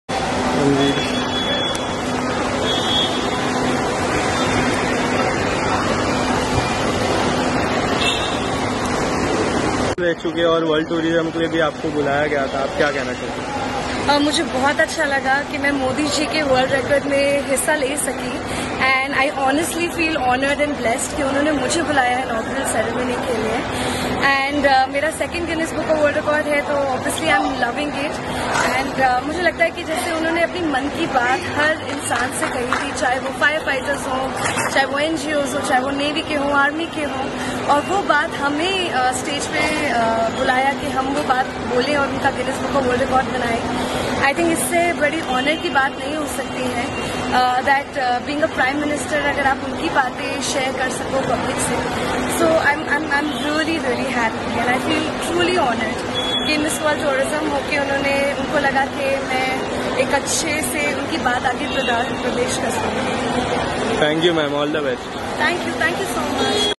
बॉलीवुड एक्ट्रेस Ishika Taneja को गुजरात से लौटते वक्त मुंबई एयरपोर्ट पर किया गया स्पॉटवीडियो देखिये
बॉलीवुड एक्ट्रेस Ishika Taneja को गुजरात से लौटते वक्त मुंबई एयरपोर्ट पर किया गया स्पॉट, वीडियो देखिये